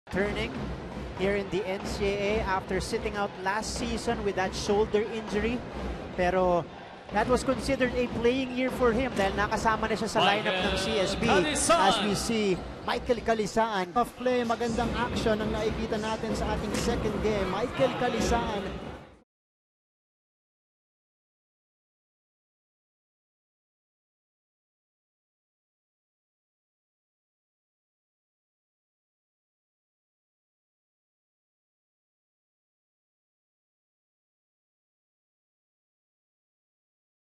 NCAA 94 MB: Michael Calisaan carves through Benilde defense for nifty reverse | SSC-R | Best Plays
Michael Calisaan powers his way inside for a reverse layup.